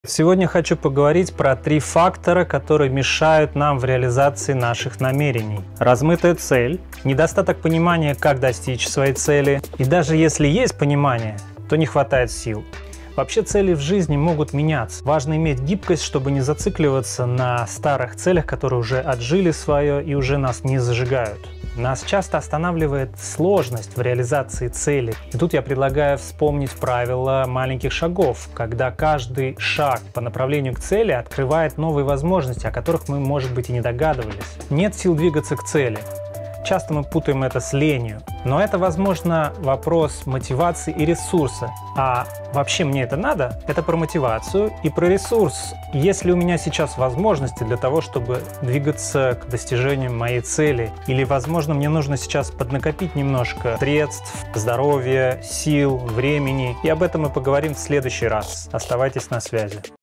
3 ФАКТОРА, МЕШАЮЩИХ РЕАЛИЗОВЫВАТЬ НАШИ НАМЕРЕНИЯ
Почему не все наши желания исполняются, как начать процесс формирования своей реальности.

Юрий Бодхе